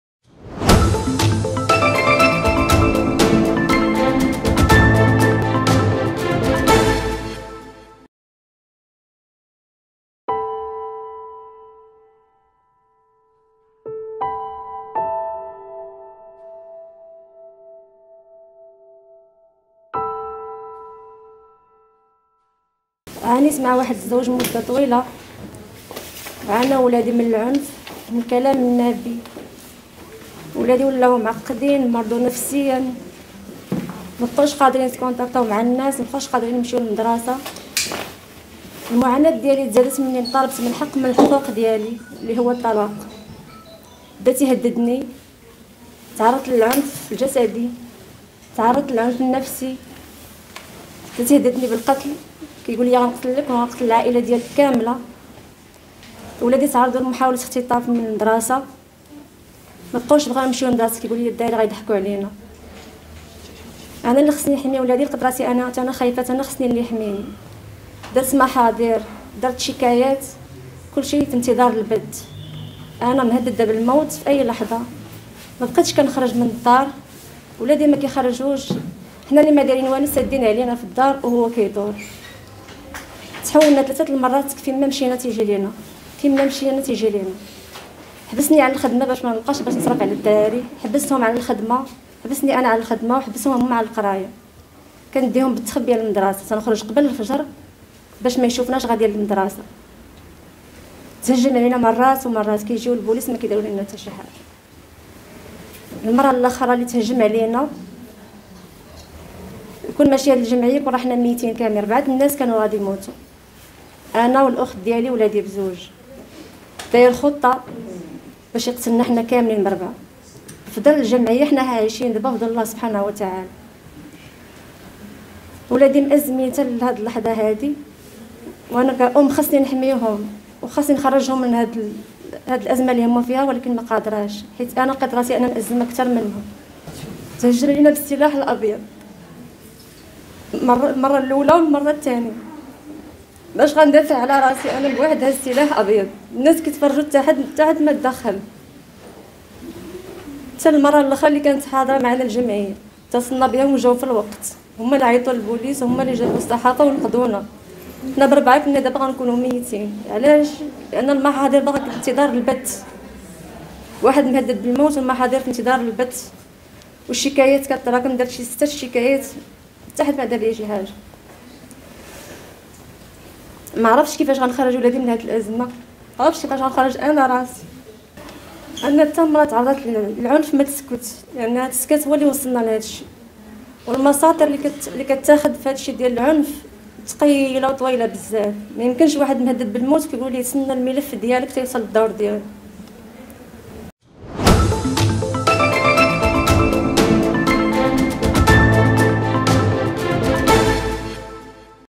أولادي تعقدو من كثرة الضرب اللي كنتلقاه من راجلي
هي شهادة صادمة من خذه المرأة التي تتعرض للعنف باستمرار.
وتتعرض العديد من النساء بالمغرب من شتى المستويات الثقافية والاجتماعية، للعنف، إما من قبل أزواجهن أو أقاربهن أو الغير في الشارع العام، بل وحتى في وسائل التواصل الاجتماعي.

# أخبار_عاجلة